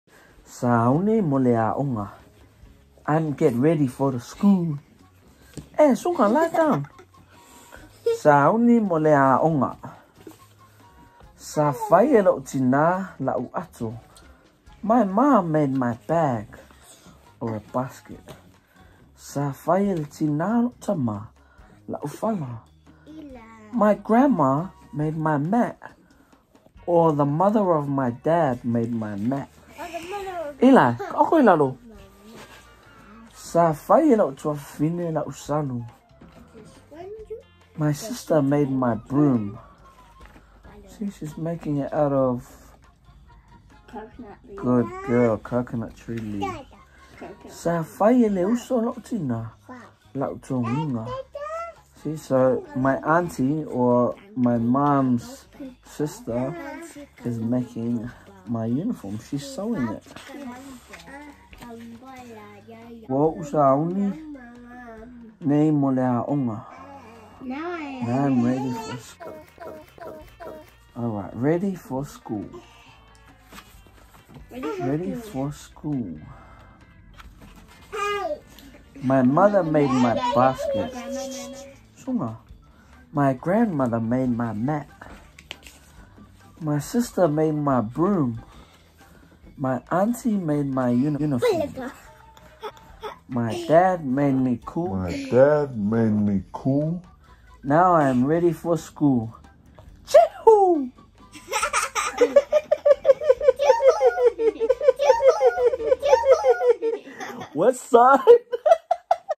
Sāuni mō le Ā’oga
Story time with your child’s favourite uncle.

Book: Sāuni mō le Ā’oga
By Emma Kruse Va'ai